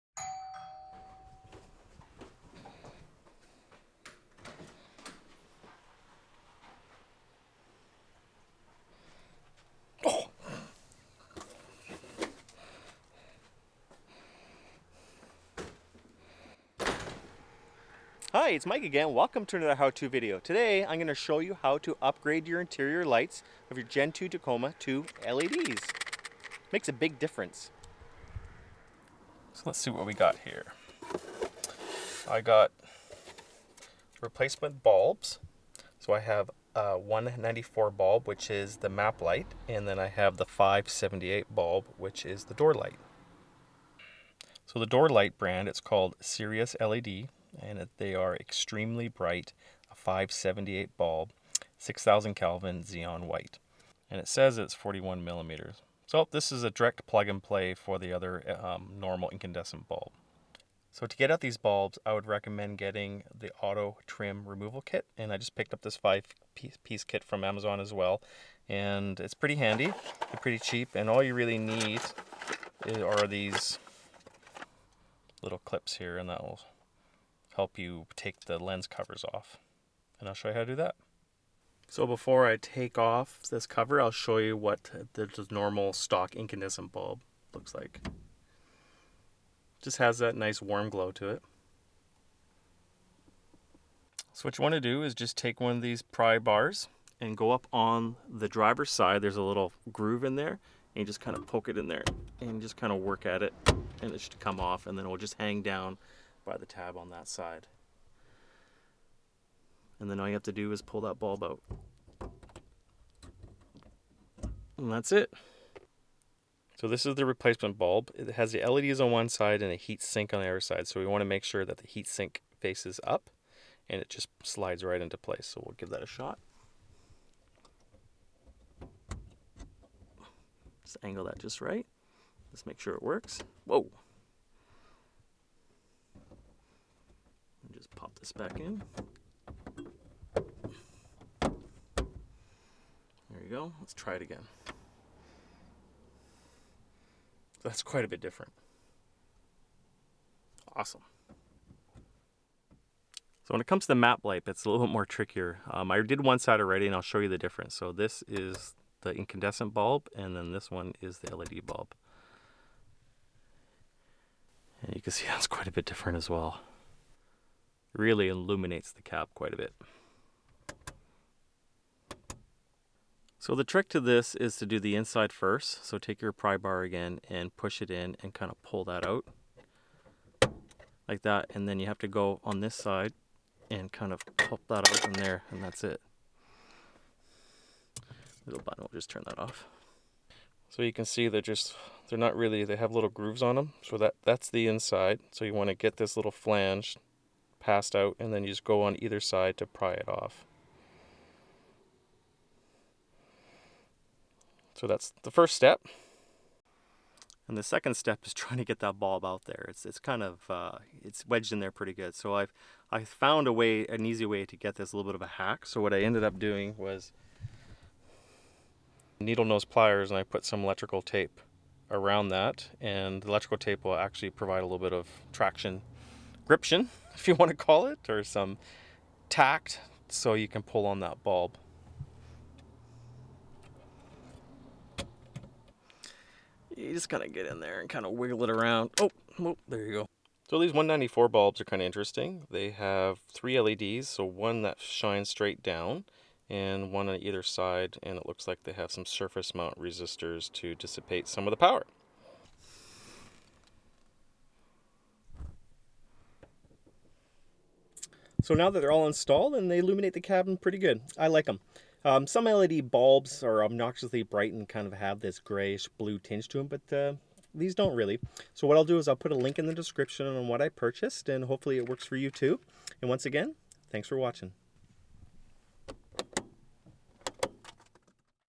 Toyota Tacoma Gen 2 (2005-2015) Interior light LED Conversion // A must mod and it's EASY to do!
How to convert your interior OEM incandescent bulb to modern LED lights.
Generation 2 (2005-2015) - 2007 Toyota Tacoma shown in video.

Sometimes it's just  a lucky guess on what LED's to purchase but these seems to be just right for the application and its relatively easy to swap out.

A dramatic improvement! Perfect for camping and Overlanding. This really helps seeing inside the cab! LED's also draw a fraction of the power of incandescent bulbs!

MAP Light

DOOR Light

Body Trim Removal Tools